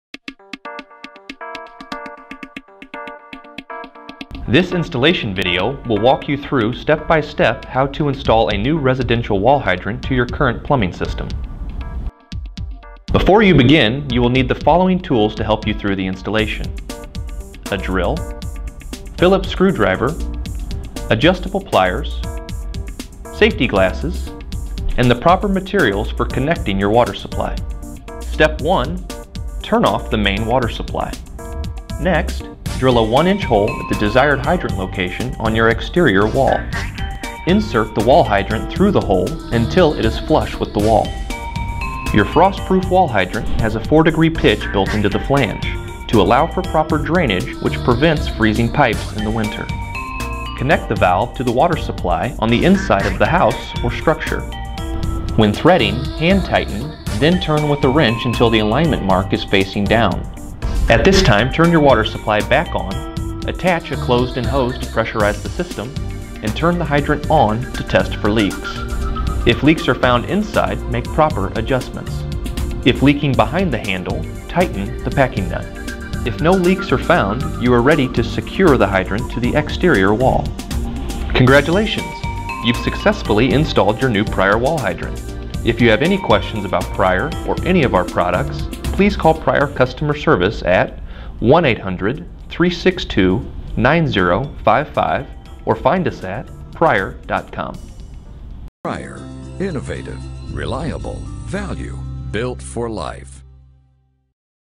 P-164T residential outdoor faucet installation
PRIER Products residential outdoor faucet (hose bib/hydrant) with 3/4"MIP X 1/2"FIP inlet step-by-step installation instructions.
Tools for the Job:
- drill
- phillips screwdriver
- adjustable pliers
- safety glasses
- connection tools

Step 1: Turn off water supply
Step 2: Drill 1" hole
Step 3: Insert wall hydrant through the hole
Step 4: Connect the valve to water supply
Step 5: Pressure test for leaks
Step 6: Adjust connection or packing nut if leaks are found
Step 7: Secure hydrant to exterior wall

For more information about PRIER Products and the P-164 residential wall hydrant, go